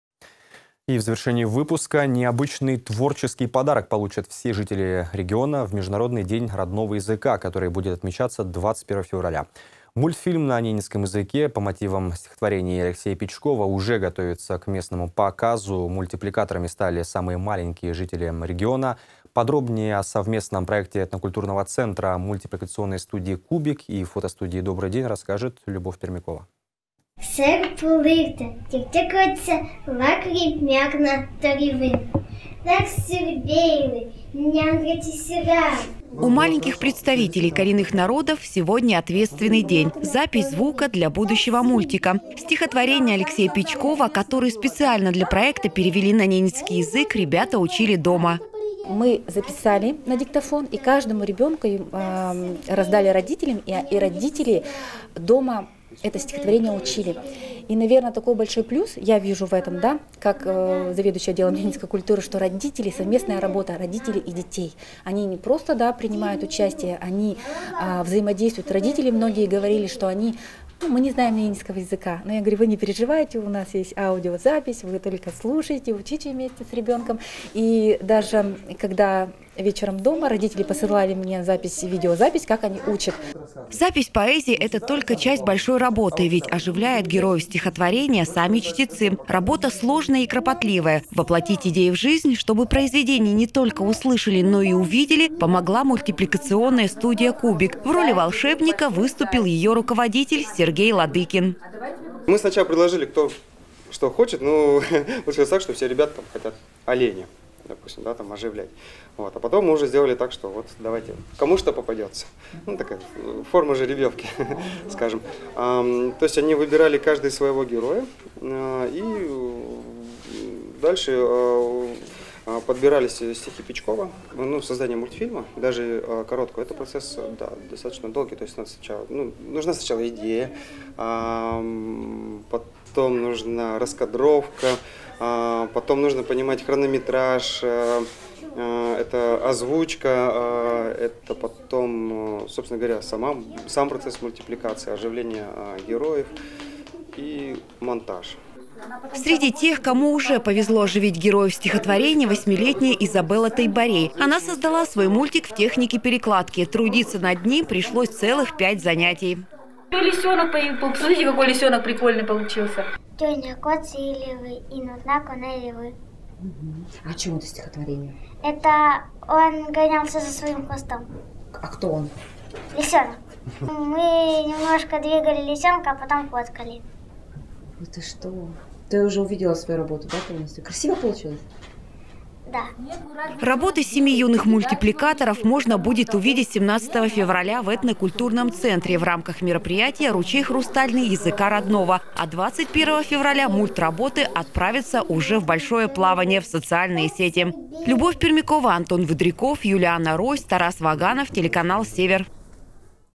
Юные нарьянмарцы пробуют себя в роли мультипликаторов
Необычный творческий подарок получат все жители Ненецкого округа в Международный день родного языка, который будет отмечаться 21 февраля. Мультфильм на ненецком языке по мотивам стихотворений Алексея Пичкова уже готовится к прокату. Мультипликаторами стали юные жители региона.